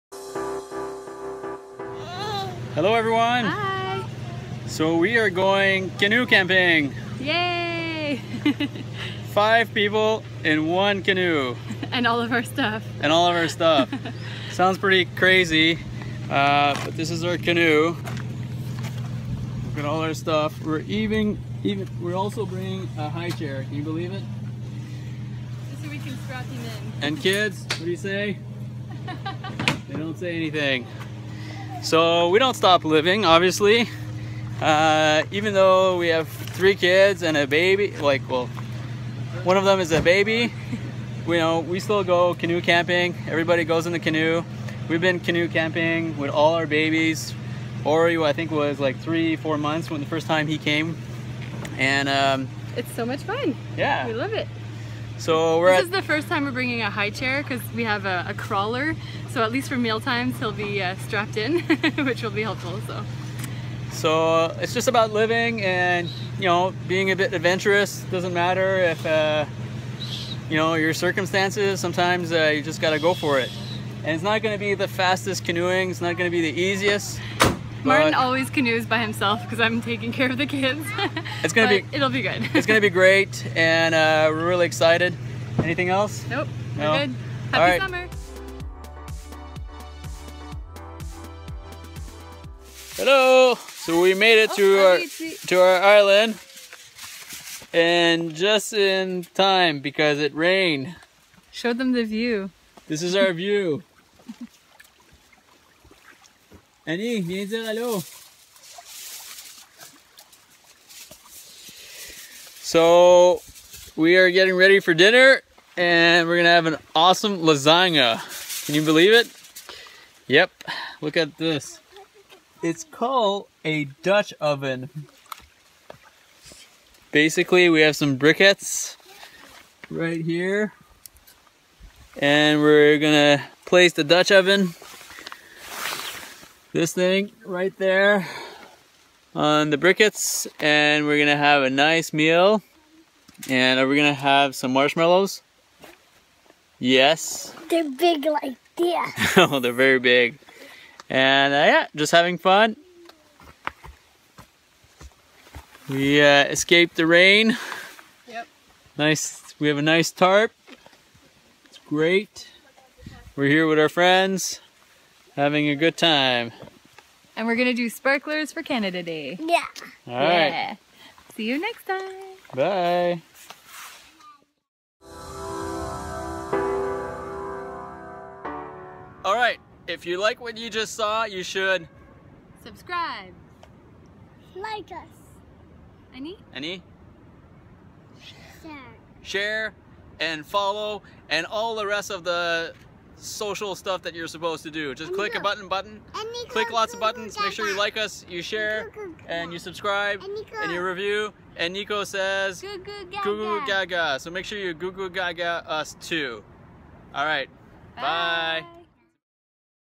Canoe Camping With Family | Join Our Adventure! | Family Camping Vlogs
Three kids and two adults in one canoe. We like to take the whole family camping so join us on our adventure.

for more!